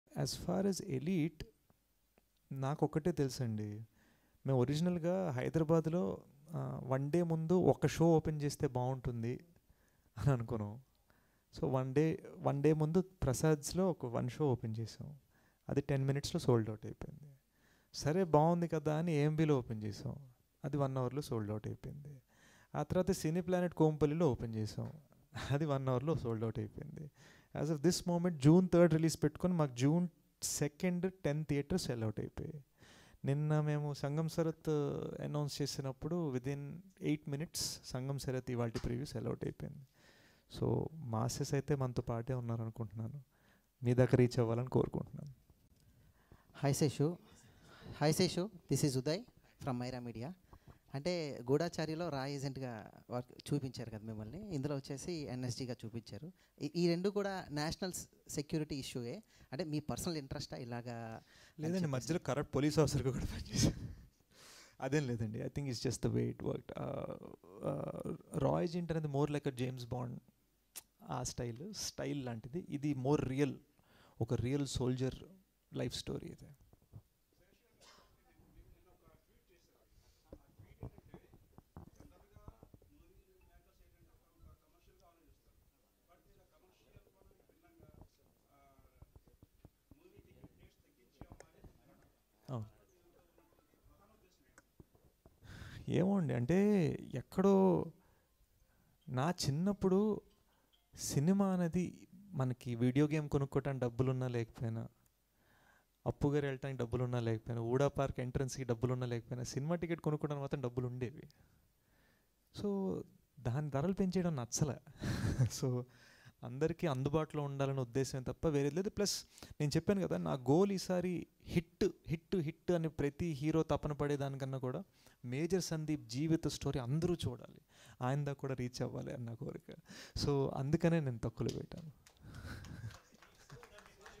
మేజర్ సినిమా హిట్ అవ్వాలని కోరుకోవట్లేదు రా : Adivi Sesh Shocking Comments On Major Movie
Watch #
Welcome to Mana TFI TV Channel
Telugu latest movie teasers, trailers, fun videos and exclusive interviews are available in our channel.